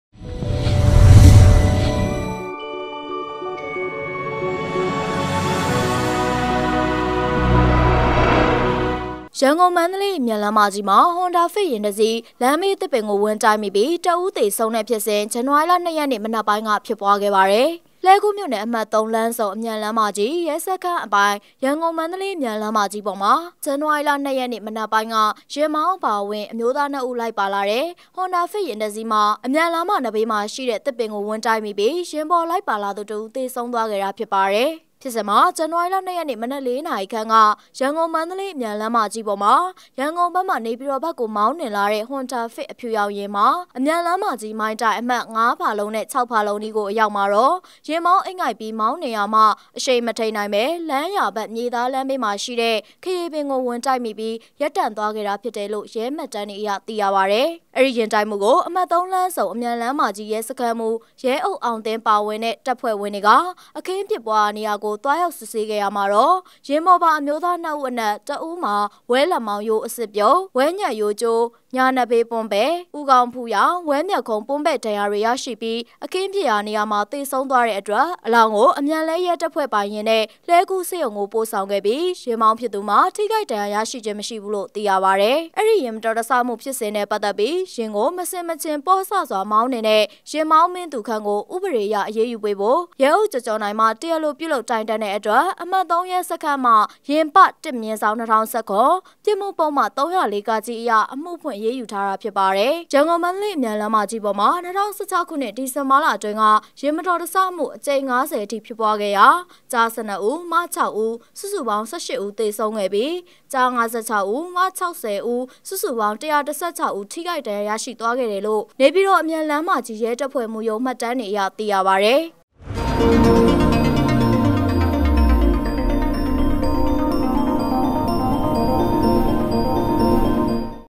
ရန္ကုန္ - မႏၱေလးအျမန္လမ္းမၾကီးတြင္ ဟြန္ဒါဖစ္ယာဥ္တစ္စီး လမ္းေဘးသစ္ပင္အား ၀င္တုိက္မိျပီး
ရန္ကုန္ - မႏၱေလးအျမန္လမ္းမၾကီးမွာ ဟြန္ဒါဖစ္ယာဥ္တစ္စီး လမ္းေဘးသစ္ပင္ကို ၀င္တုိက္မိျပီး လူတစ္ဦး ေသဆုံးတဲ့ျဖစ္စဥ္ ဇန္န၀ါရီလ၂ရက္ေန႔ နံနက္ပုိင္းက ျဖစ္ပြားခဲ့ပါတယ္။